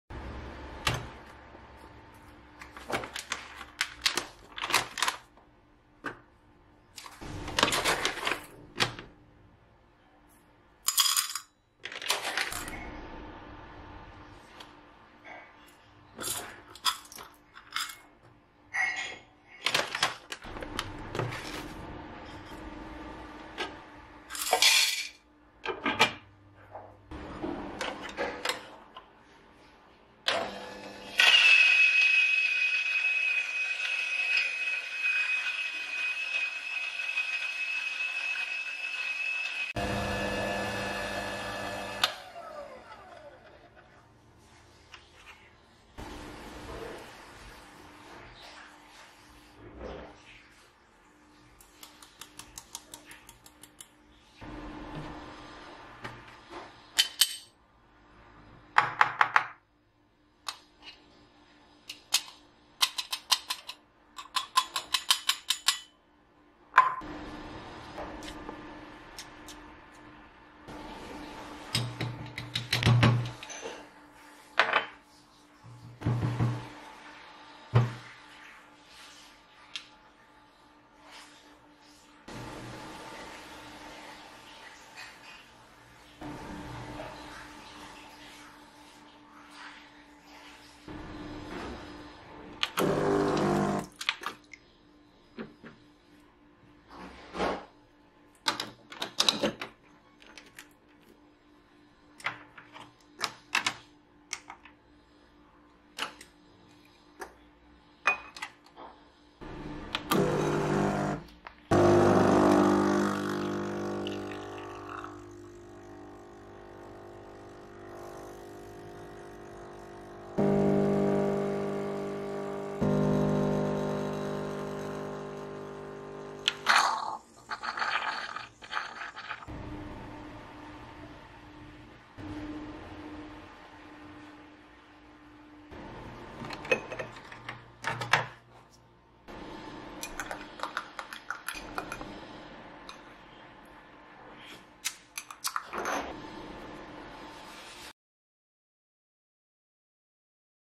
making double shot espresso ASMR
akhirnya nemu juga masalah di grinder LD019, ternyata burr atasnya tidak flat alias gelombang dan setelah di tweek supaya rata, bisa juga bikin espresso 1:2